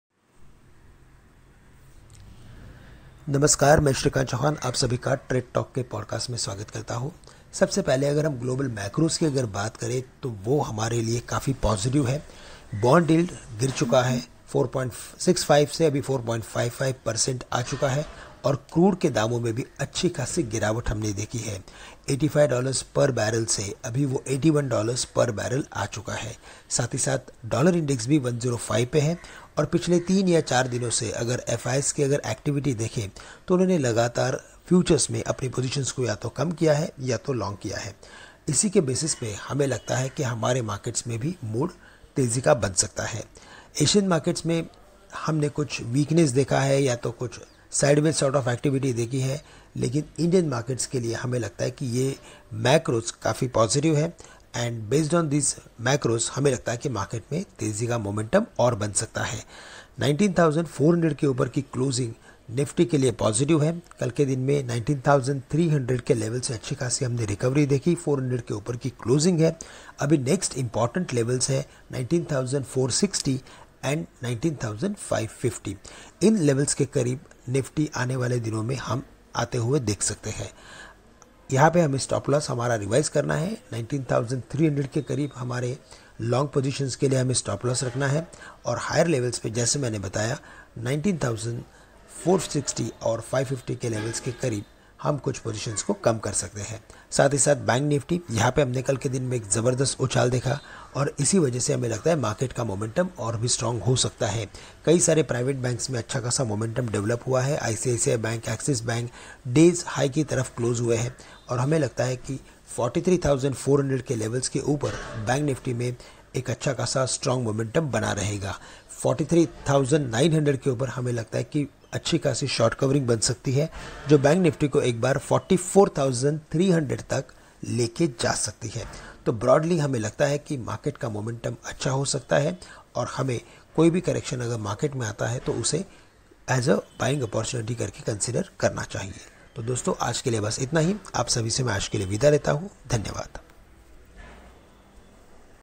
निफ्टी 19,550 एक प्रमुख रेसिस्टेंस स्तर; बैंक निफ्टी के लिए 43,900 अहम स्तर है
श्रीकांत चौहान, रिसर्च हेड, कोटक सिक्योरिटीज, लेकर आएंगे हमारा डेली ट्रेड टॉक पॉडकास्ट अब हिंदी में भी |

हमारे दैनिक ट्रेड टॉक पॉडकास्ट में श्रीकांत चौहान कहते हैं, निफ्टी 19,550 दिन के लिए एक प्रमुख रेसिस्टेंस स्तर होगा, जबकि निफ्टी 19,150 एक प्रमुख सपोर्ट स्तर के रूप में कार्य करेगा। निफ्टी और बैंक निफ्टी के लिए यहां पूरी ट्रेडिंग रणनीति प्राप्त करें।